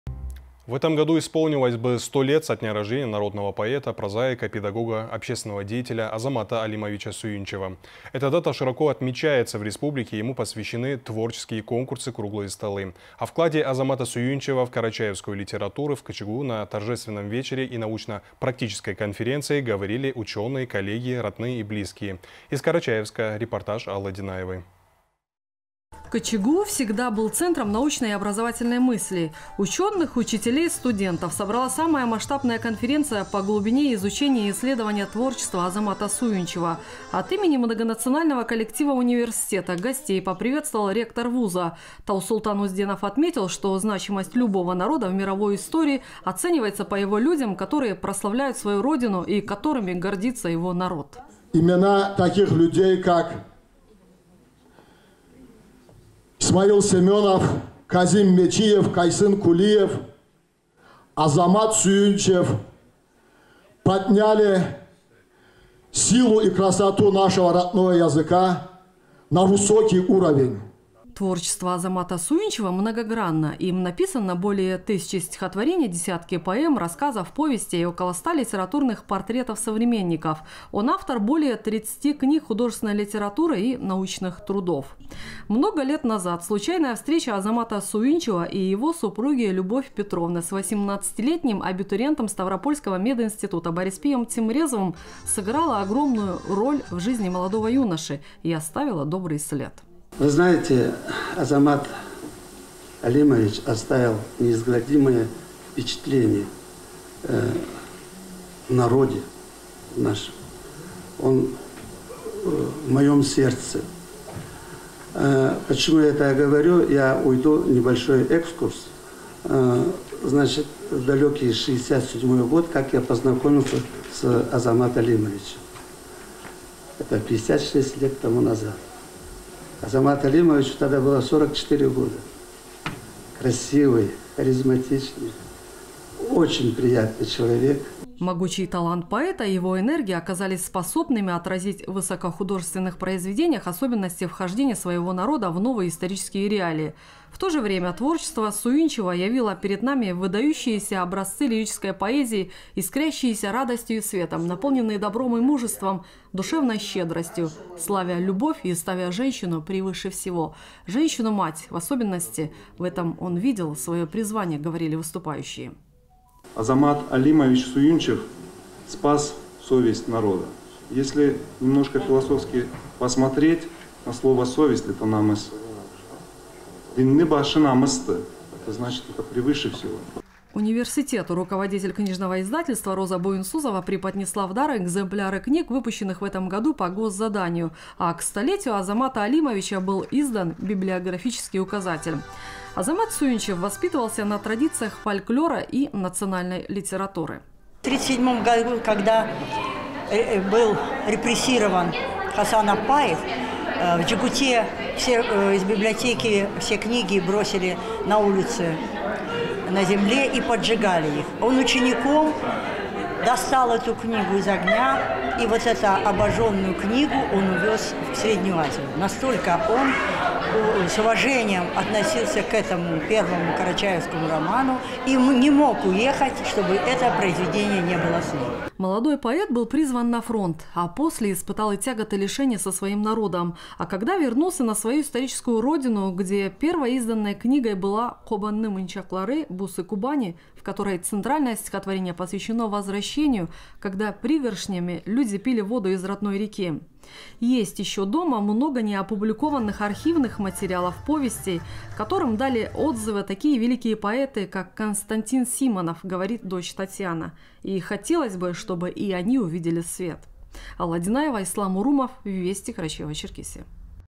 В КЧГУ прошел торжественный вечер к 100-летию со дня рождения Азамата Суюнчева
В этом году исполнилось бы 100 лет со дня рождения народного поэта, прозаика, педагога, общественного деятеля Азамата Алимовича Суюнчева. Эта дата широко отмечается в Республике, ей посвящены творческие конкурсы, круглые столы. О вкладе Азамата Суюнчева в карачаевскую литературу в КЧГУ, на торжественном вечере и научно-практической конференции, говорили учёные, коллеги, родные и близкие